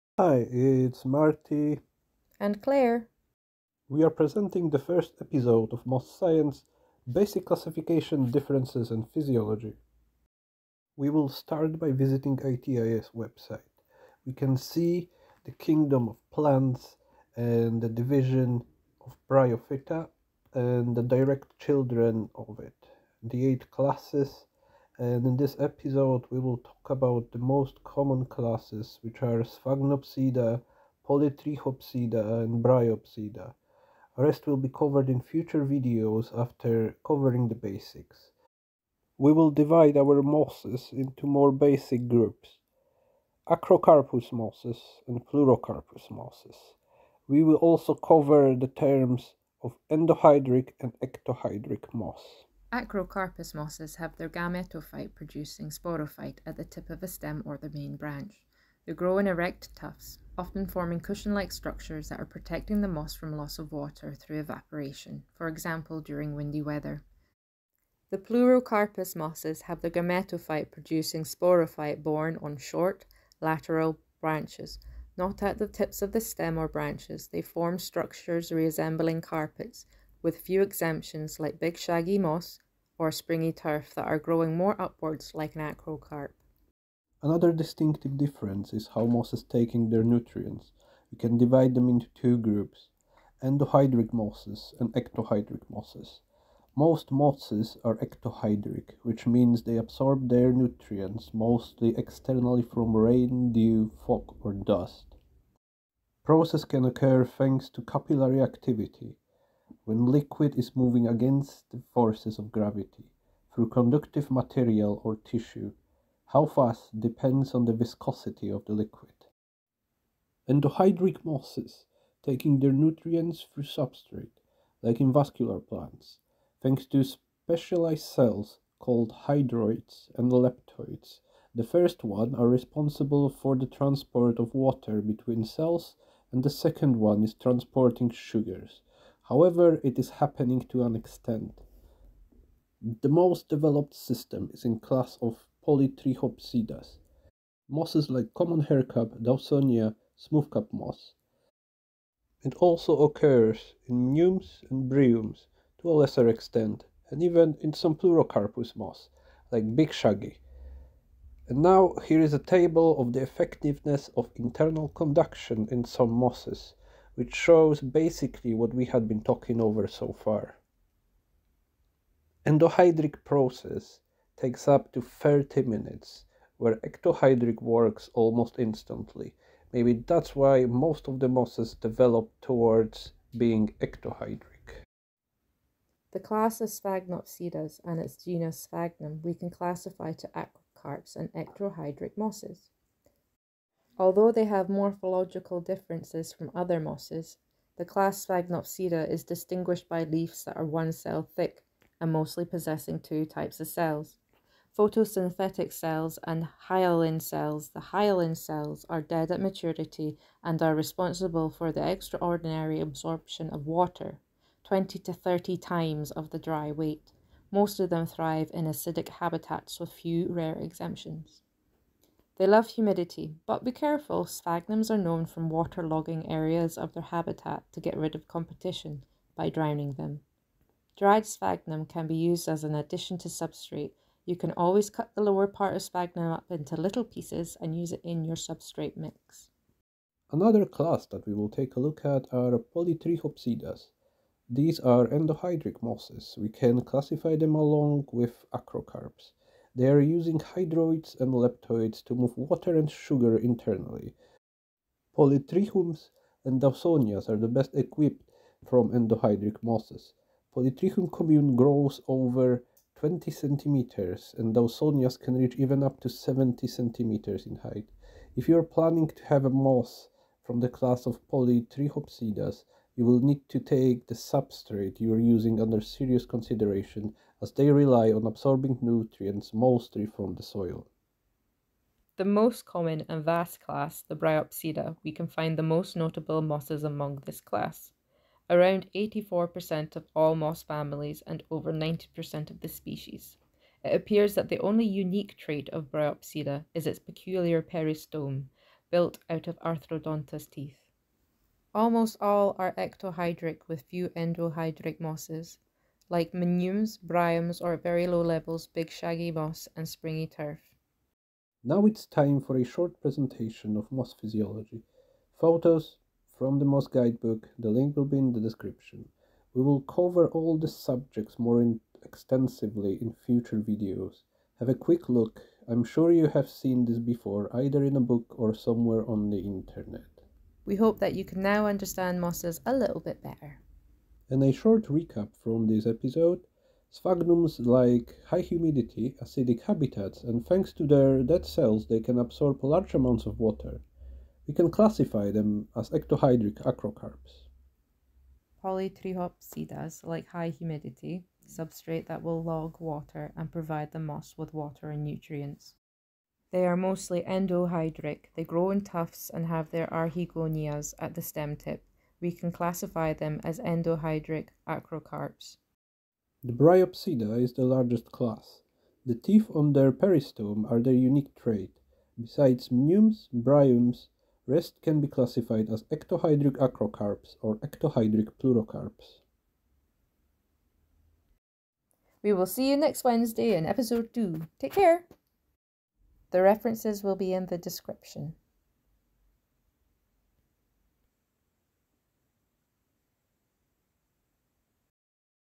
Moss Science Episode 1. Basic classification, differences and physiology of mosses.
References:

*D.H.S. Richardson "The biology of mosses"

*British Bryological Society "Mosses and Liverworts of Britain and Ireland a field guide"

*J.M. Glime "Bryophyte Ecology"

*Britannica

*Wikipedia